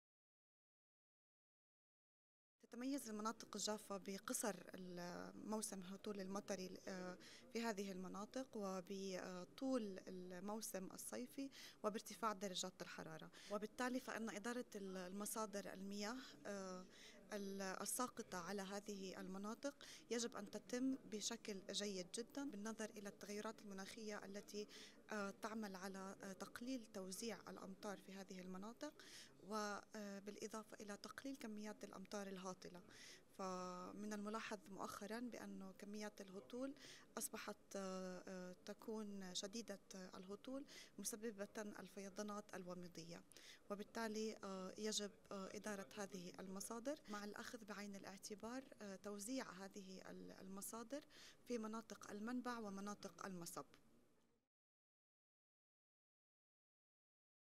Mira Haddad at the Cairo Water Week 2022
Listen to Mira Haddad the Research Associate - Spatio-temporal assessment (ICARDA) at the Digital Applications for Improved Water Resources Management technical session